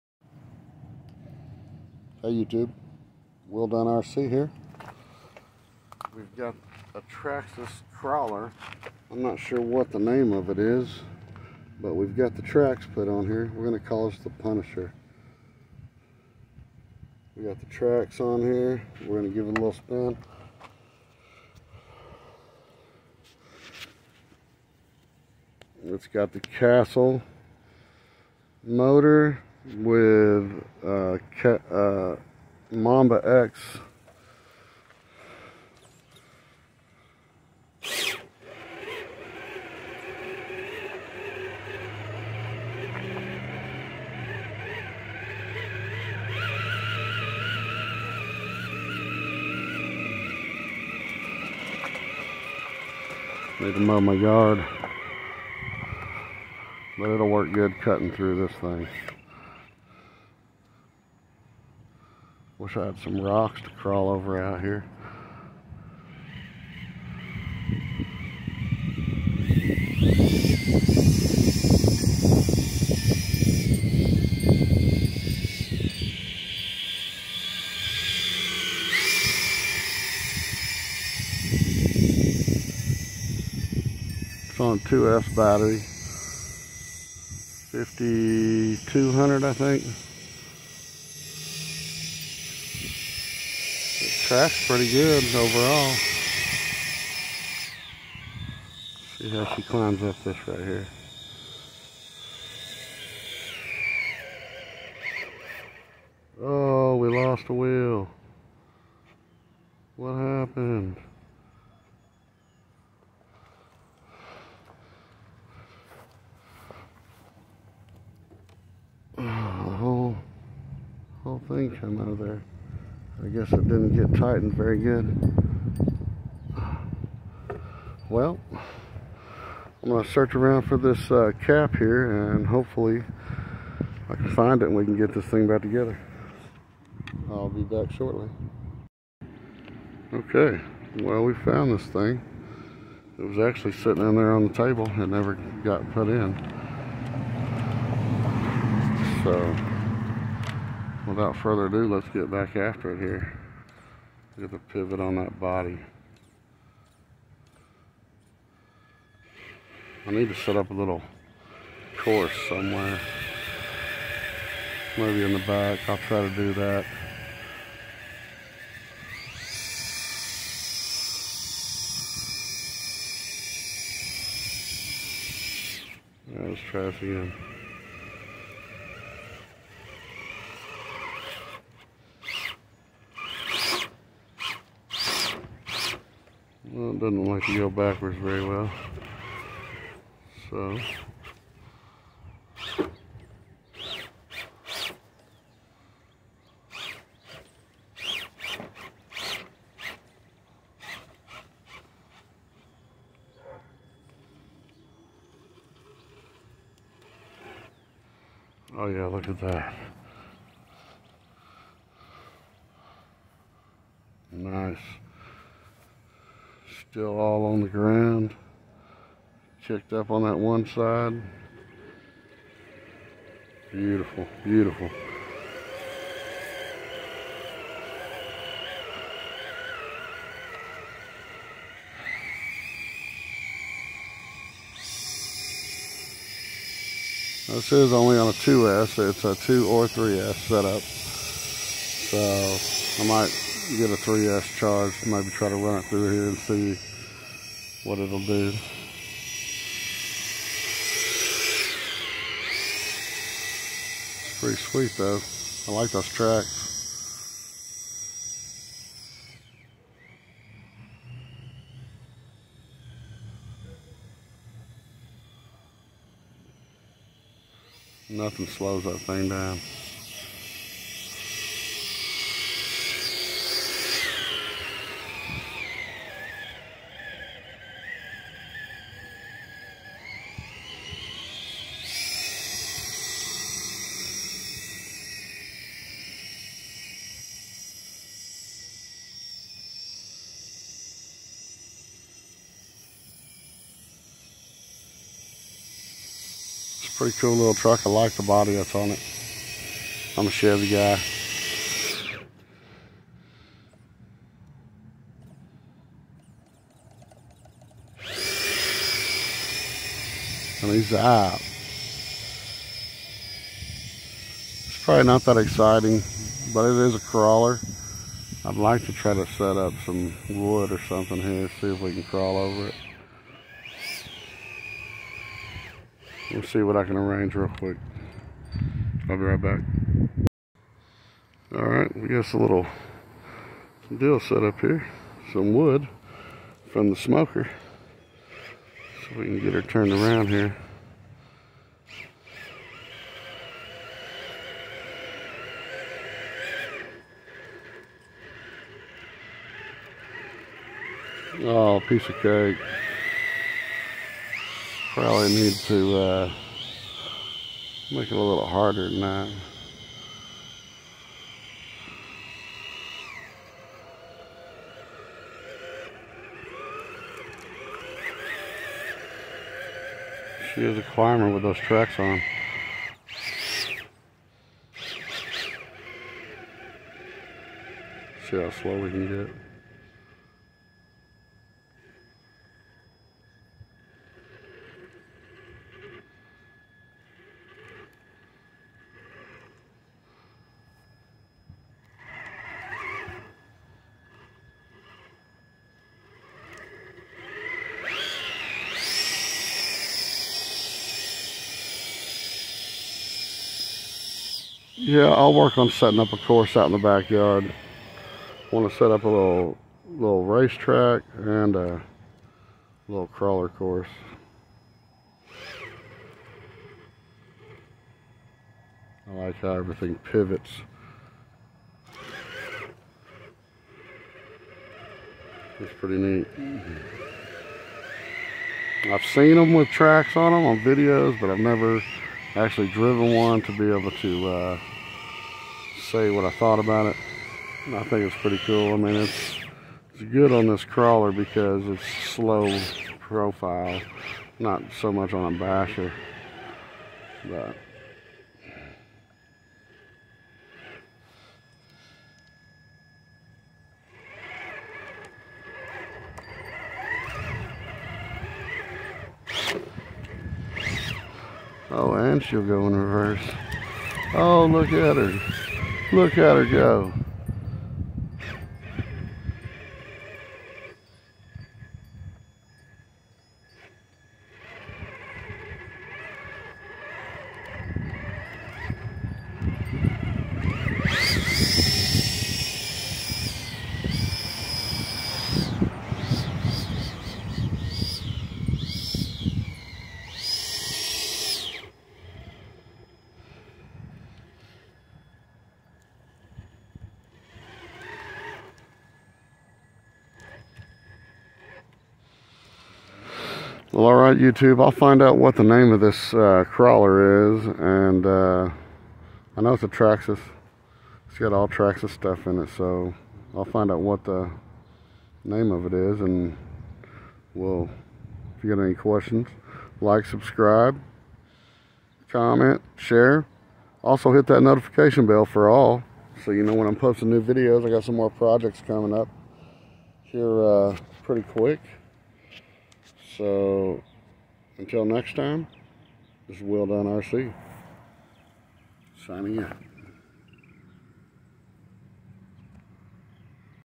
Traxxas TRX4 Sport crawler 4x4 with punisher Chevy body and trax.
Checking out this Traxxas crawler with the trax setup on her.